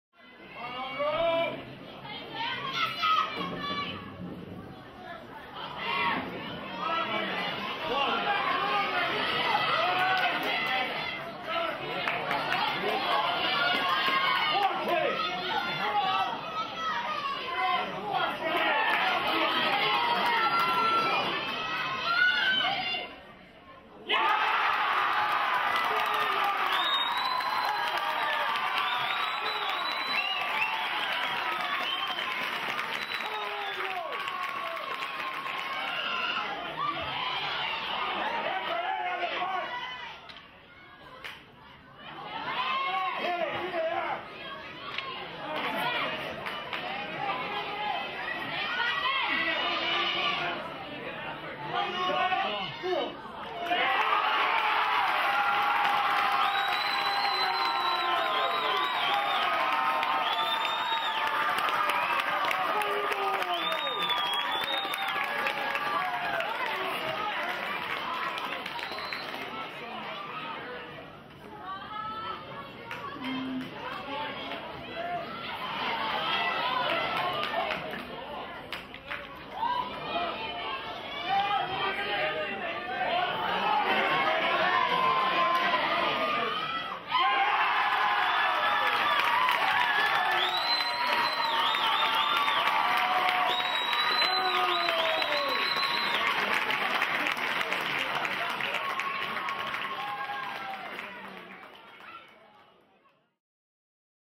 Goals Galore! Eoghan Rua v Rossa
See three goals scored by Eoghan Rua on their way to defeating champions Rossa in the 2013 NWP Ulster Senior Camogie Club Final at Owenbeg on Sunday October 27 by 4-10 to 0-3.

Video production by Jerome Quinn Media for Ulster Camogie.